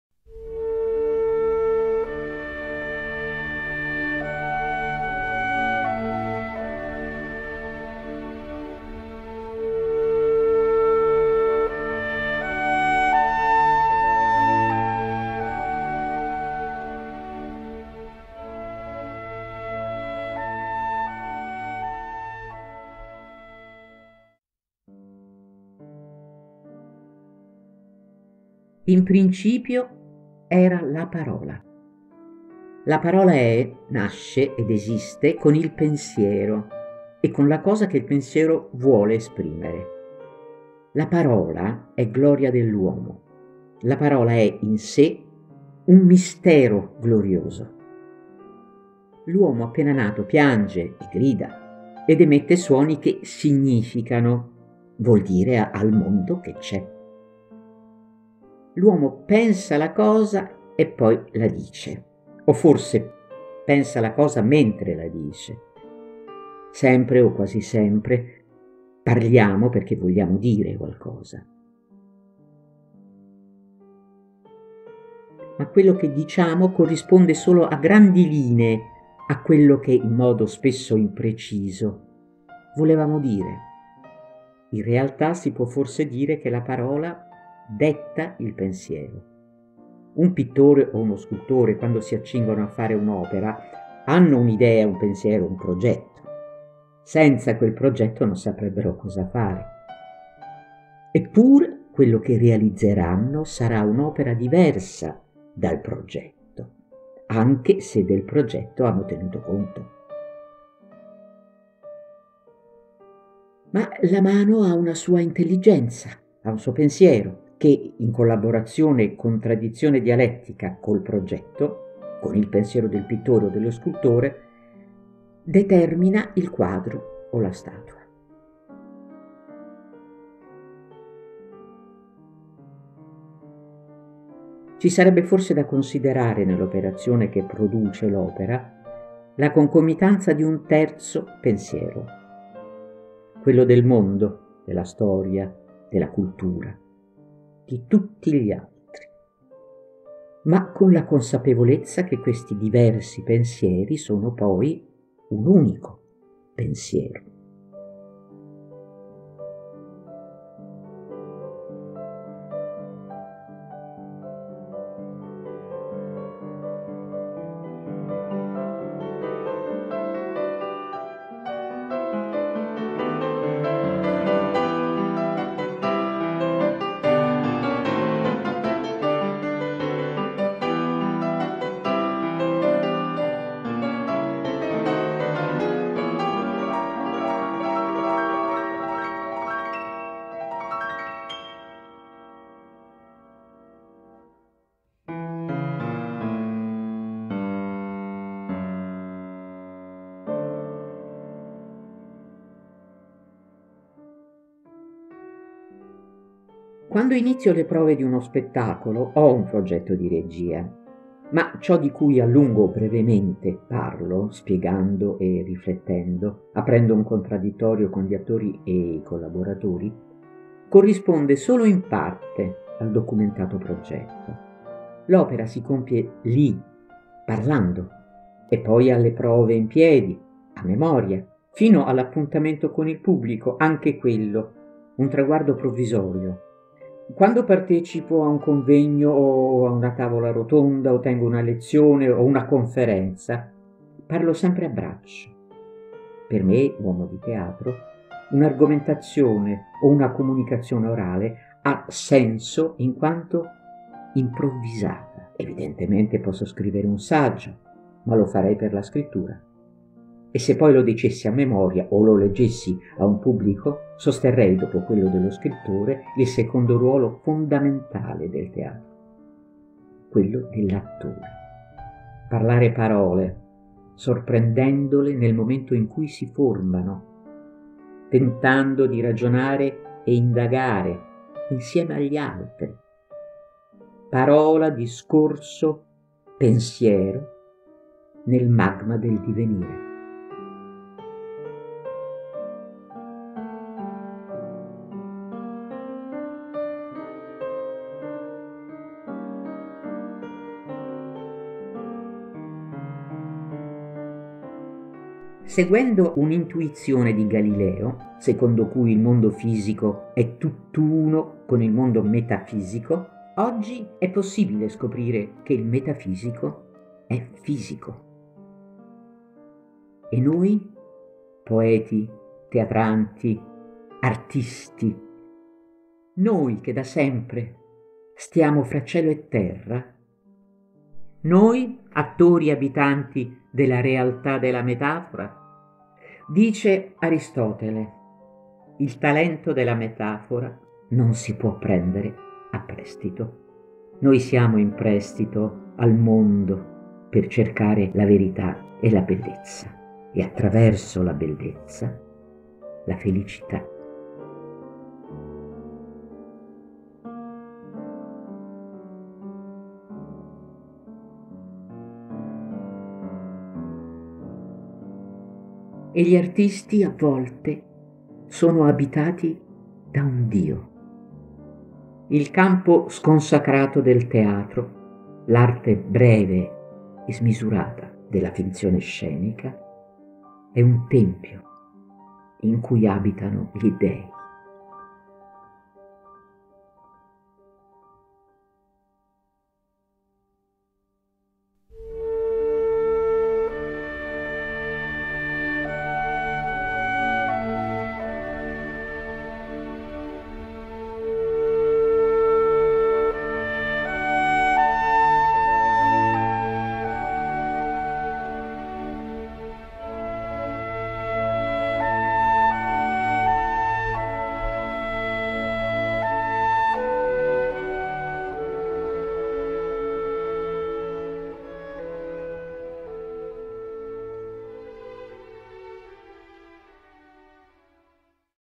PAOLA MANNONI // FISICO E METAFISICO MANNONI
🎭 TANTI MODI DI DIRE TEATRO: un omaggio a Lamberto Puggelli // Rubrica culturale a cura di Salvo Piro. 🎭

Quarta puntata // FISICO E METAFISICO: Noi che da sempre stiamo fra cielo e terra.
Dal libro Nel dolore la gioia mi parla, PAOLA MANNONI legge le parole di Lamberto Puggelli.